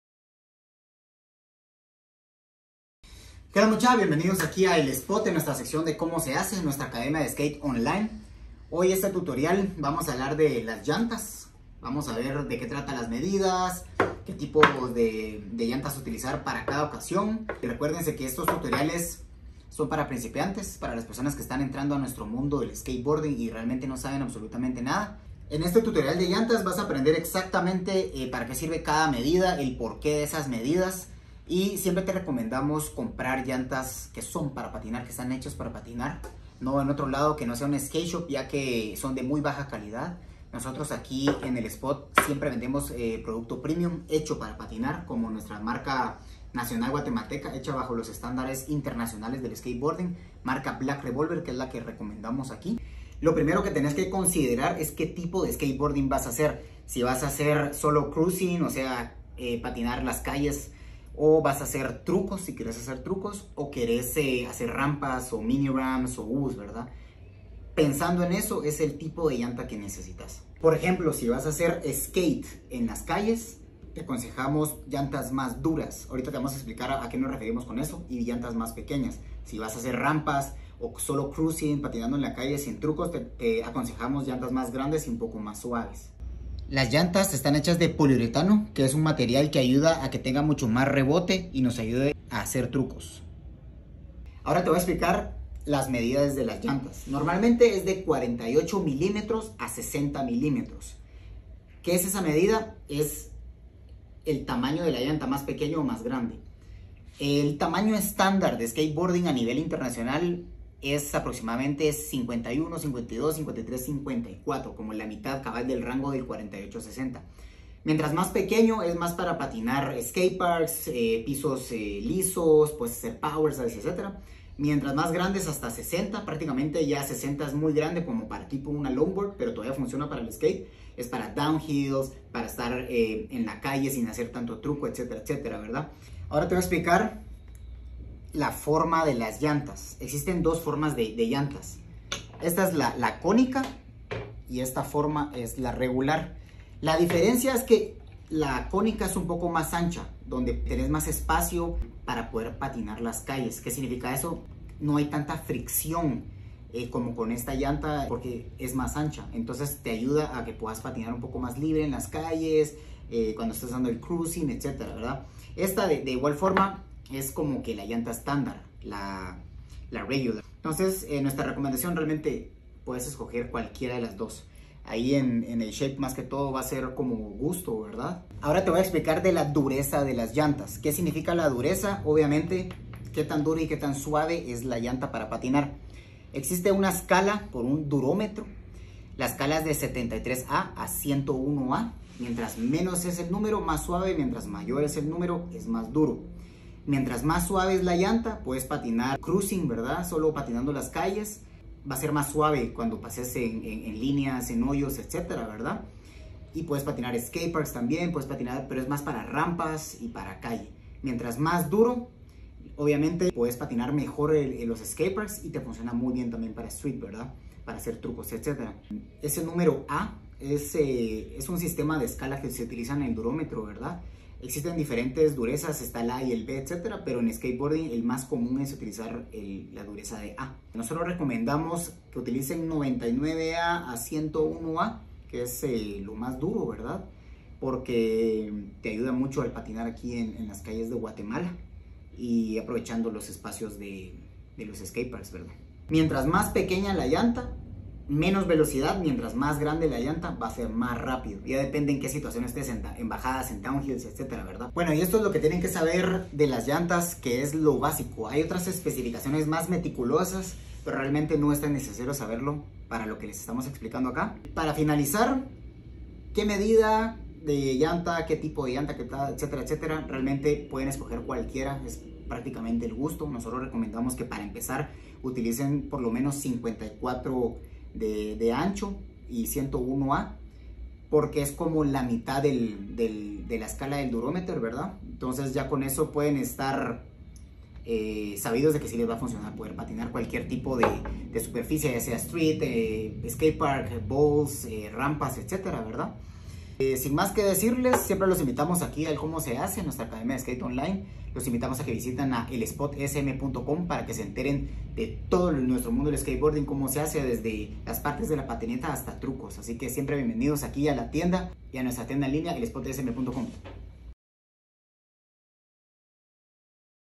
¿CÓMO SE HACE? ¿QUÉ ES Y CÓMO FUNCIONAN LAS LLANTAS DE UNA PATINETA?
¿CÓMO SE HACE?
¿QUÉ ES Y CÓMO FUNCIONAN LAS LLANTAS DE UNA PATINETA?
Con @urielruano

Así que estás empezando, vas a comprar tu primer tabla y no sabés cual es la función de las llantas ¡Perfecto!

Con este ¿Cómo se hace? Te ayudaremos a que sepás y comprendás todo desde el inicio en nuestro mundo del skate.

*Este tutorial es de info de las llantas, si querés saber de todas las partes que componen la patineta o trucos podés encontrar más en nuestra sección ¿Cómo se Hace? que es nuestra Academía de Skate Online y todos nuestros productos en elspotsm.com